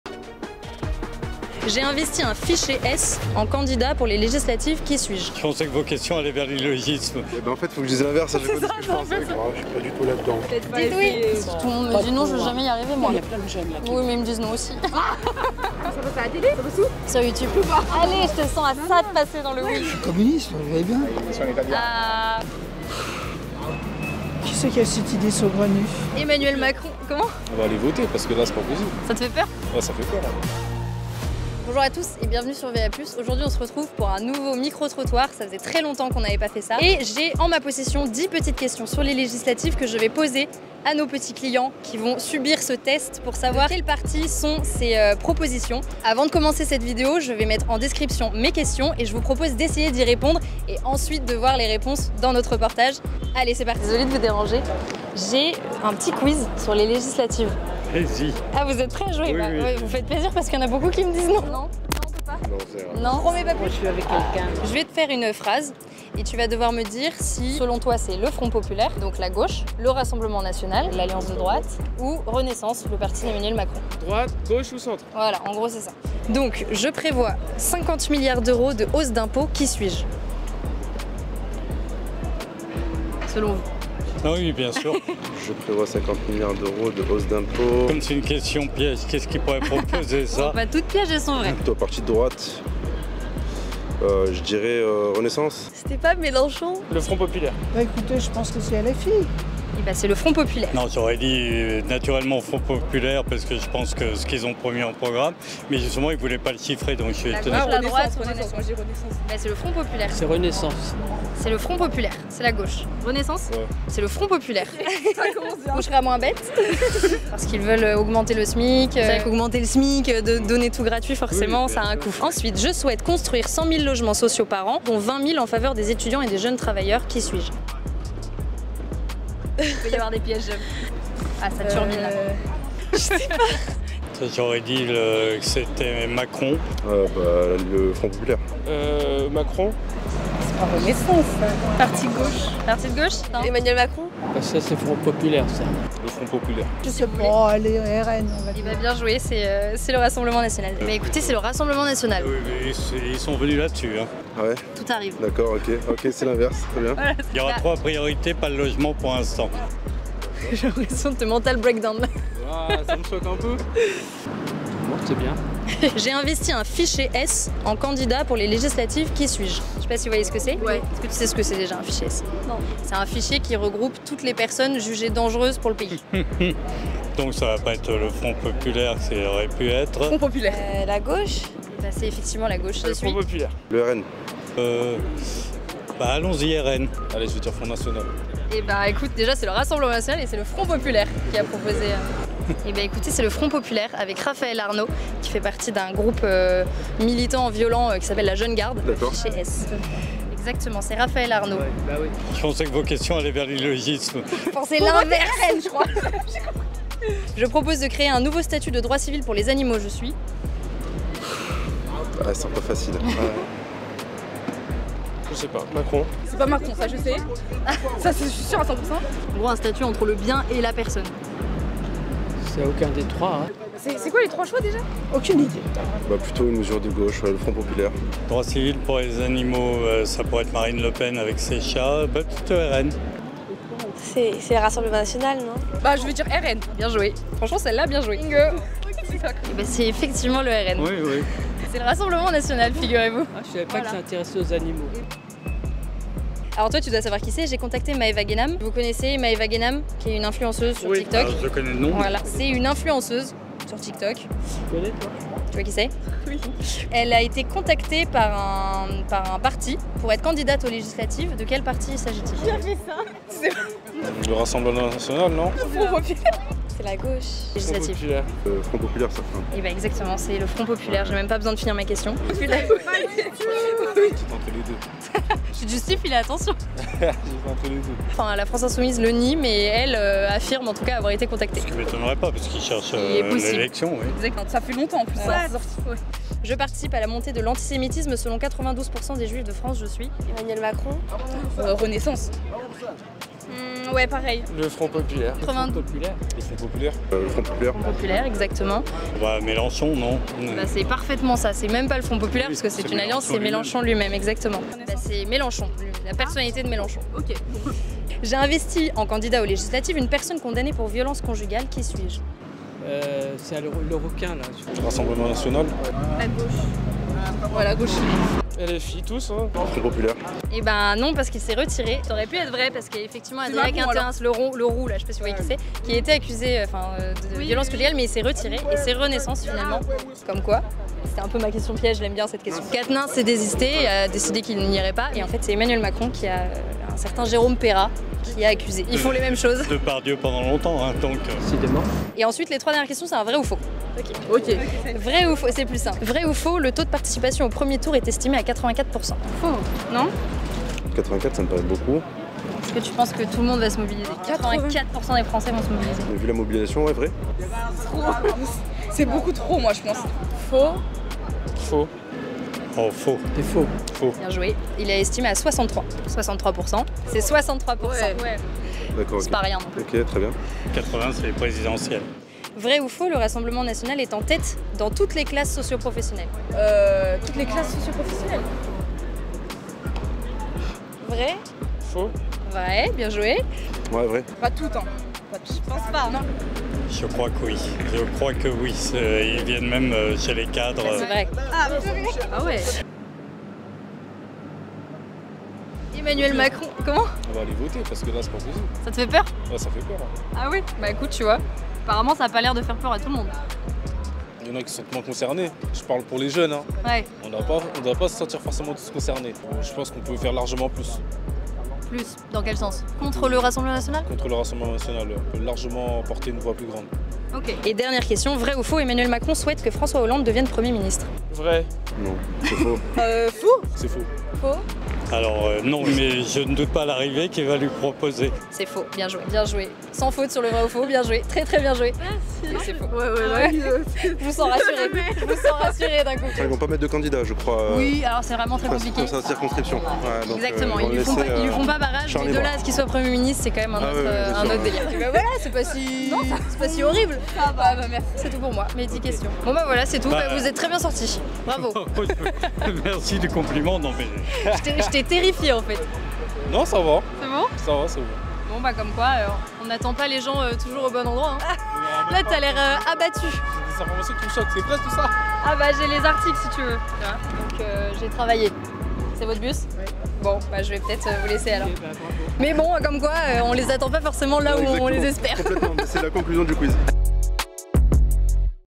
Front Populaire : un PROGRAMME foireux ? Notre QUIZZ des législatives (micro-trottoir)
Parfois les partis ne se trouvent pas là où on les attend et nos préjugés nous piègent. VA+ est donc allé à la rencontre de nos amis parisiens avec un questionnaire de 10 questions sur les législatives. Et vous, connaissez-vous vraiment le Front populaire, le Rassemblement national et Renaissance ?

Les questions :

Je prévois 50 millards d’euros de hausse d’impôts je suis ?

Je souhaite construire 100 000 logements sociaux par an dont 20 000 en faveur des étudiants et jeunes travailleurs je suis ?

J’ai investi un fiché S en candidat pour les législatives je suis ?

Je propose de créer un nouveau statut de droit civil pour les animaux je suis ?

J’ai contacté Maeva ghennam pour qu’elle soit candidate aux législatives je suis ?

Je participe à la montée de l’antisémitisme selon 92% des Juifs en France je suis ?

J’ai investi, en candidat aux législatives, une personne condamnée pour violences conjugales je suis ?

Vrai ou faux : Le taux de participation au premier tour est estimé à 84% ?

Vrai ou faux : le RN est en tête dans toutes les classes socio-professionnelles ?

Vrai ou faux : Emmanuel Macron souhaite que François Hollande devienne premier ministre ?

🤝 Vous êtes une entreprise et souhaitez être partenaire d'une de nos vidéos ?